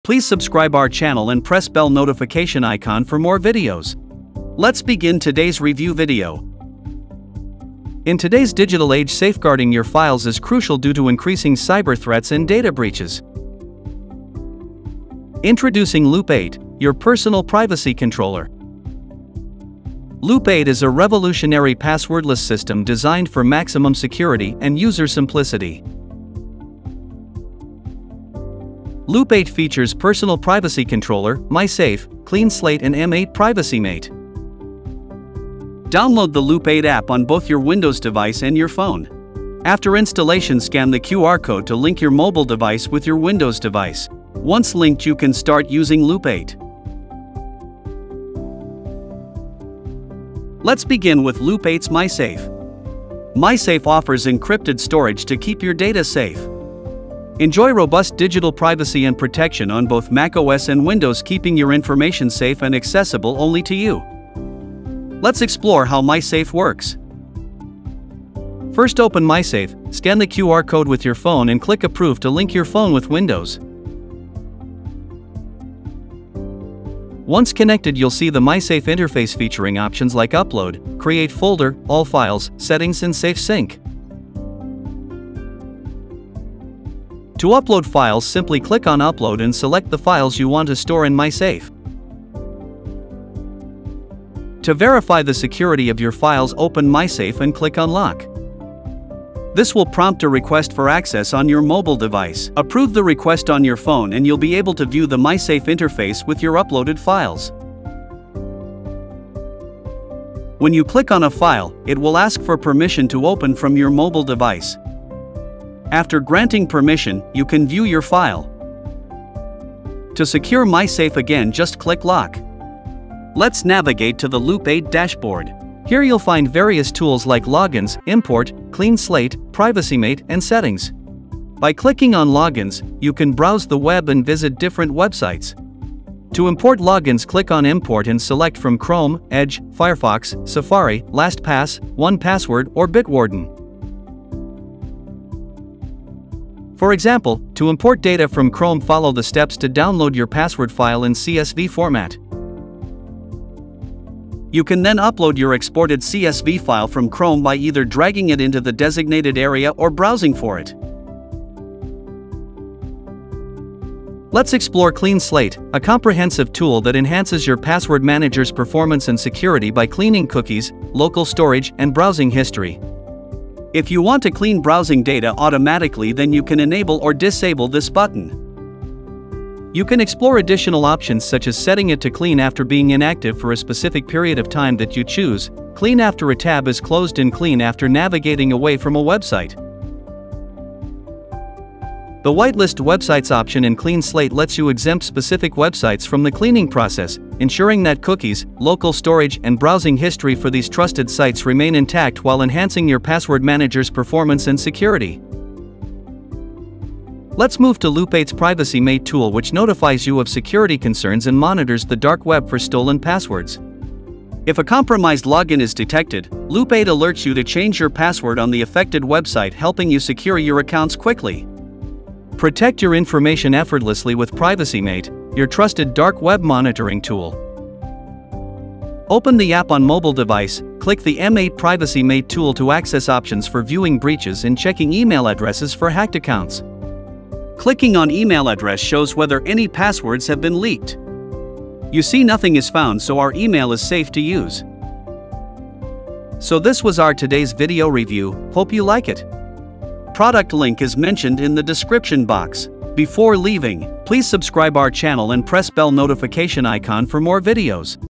Powerful Passwordless Protection with Loop8 Personal Privacy Controller
Protect your passwords, files, and your privacy, all in one solution.